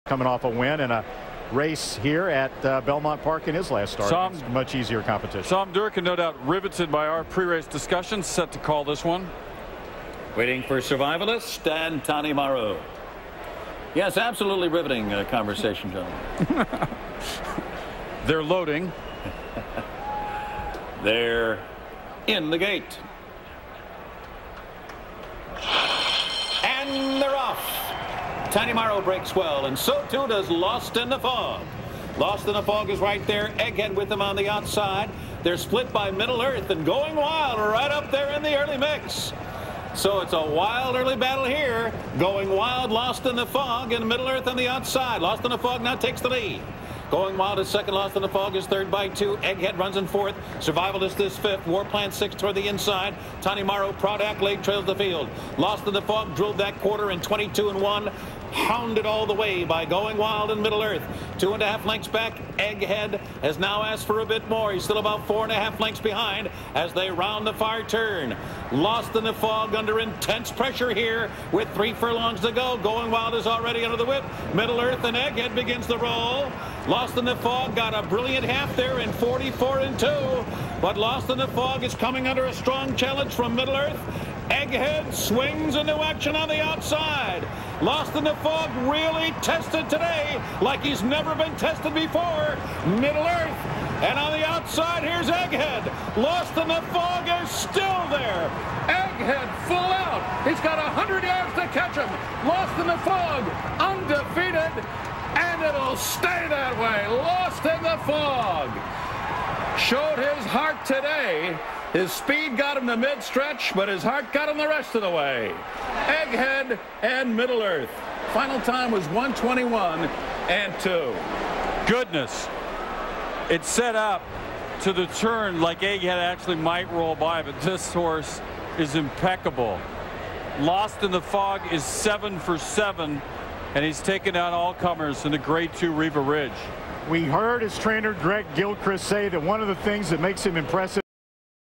2005 Riva Ridge Breeders' Cup Handicap - Lost in the Fog
ESPN's presentation of 2005 Riva Ridge Stakes.  Lost in the Fog is the winner with Egg Head second.  It's a shame that both of these great competitors are gone.